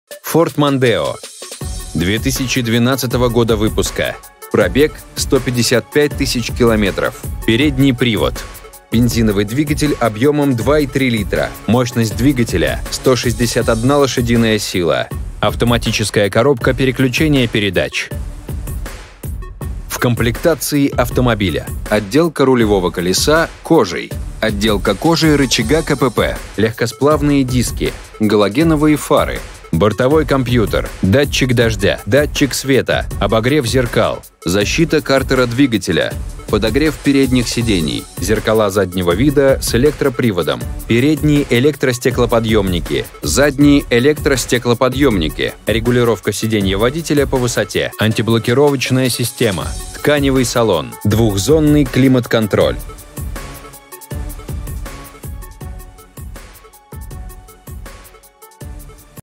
Ford Mondeo с пробегом 2012
Ford Mondeo 2012 года

Цвет - черный
Двигатель - 2.3 л./ бензин
Мощность - 161 л.с.
КПП - автомат
Привод - передний